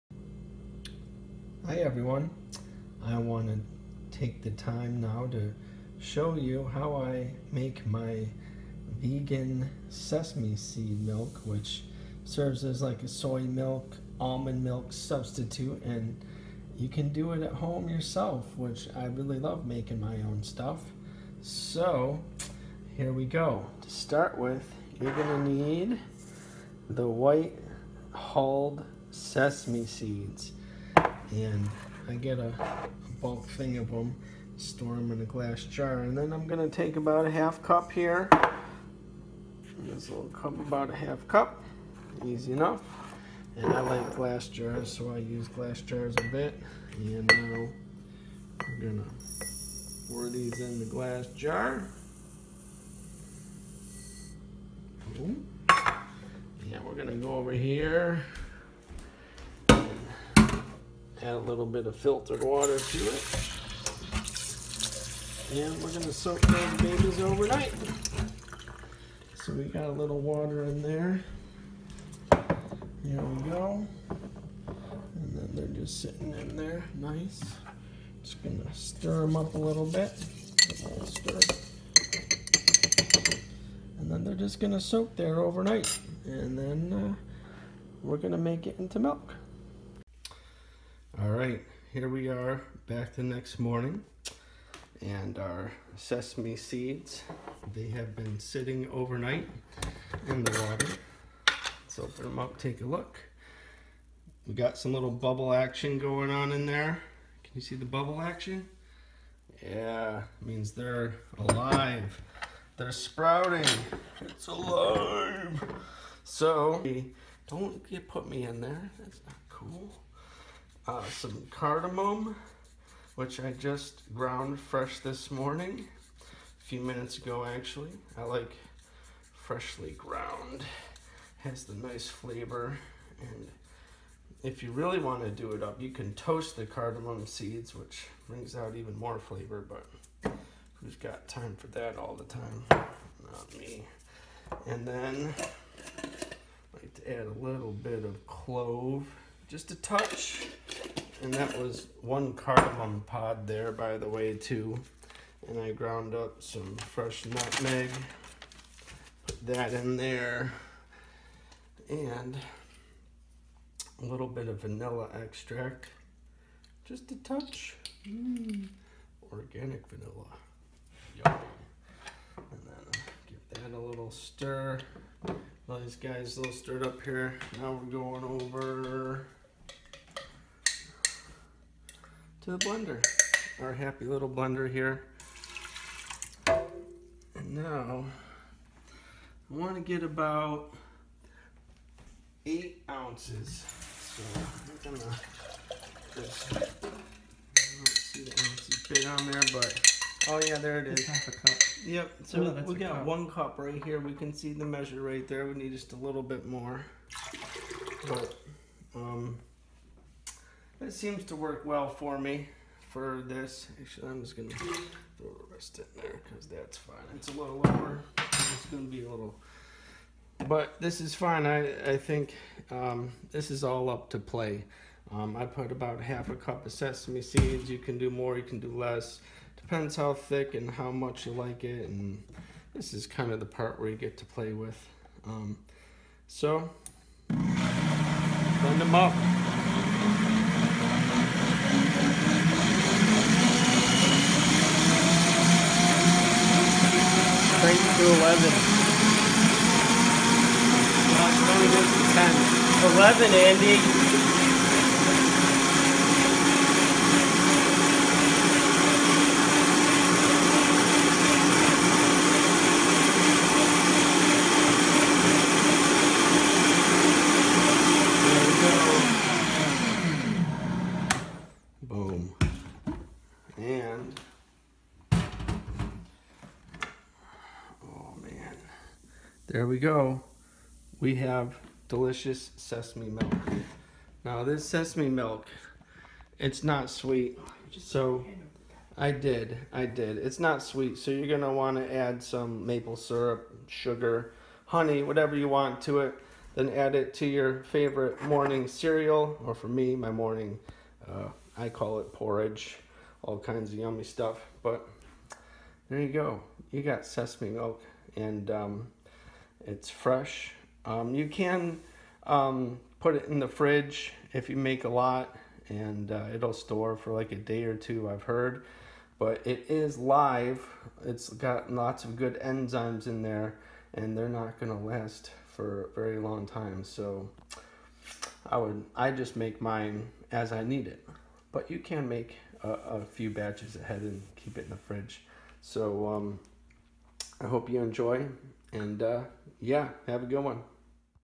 DIY vegan sesame seed milk
How to make a vegan &quot;milk&quot; at home, for the DIYers out there, using sesame seeds. Delicious and nutritious!